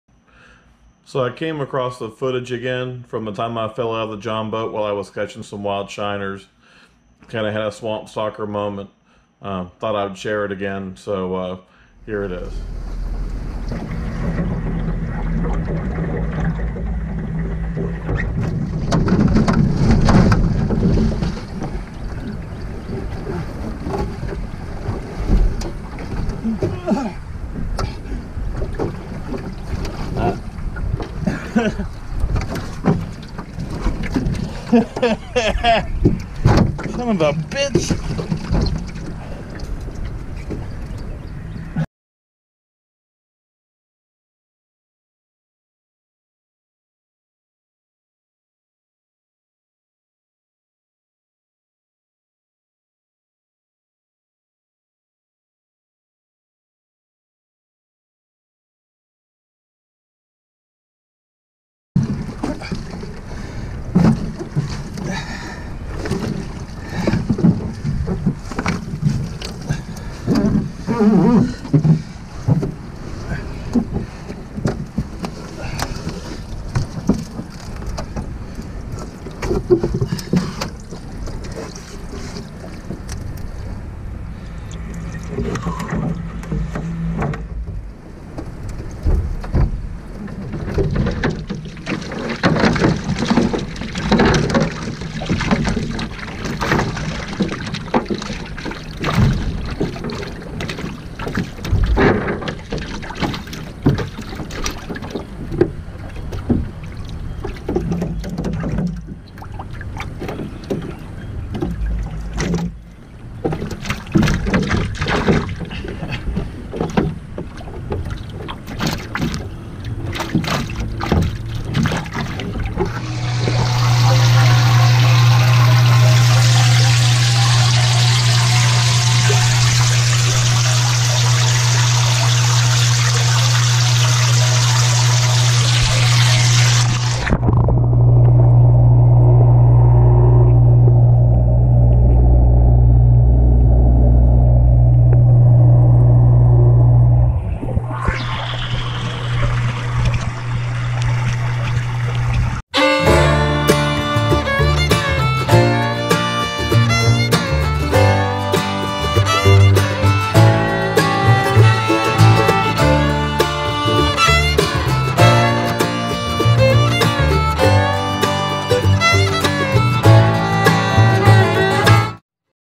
Flashback: I fell out of my boat
A couple of years ago I fell out of my jon boat while I was filming a "how to catch wild shiners" video.  I found a little of that footage in my Google drive and thought I would share it again.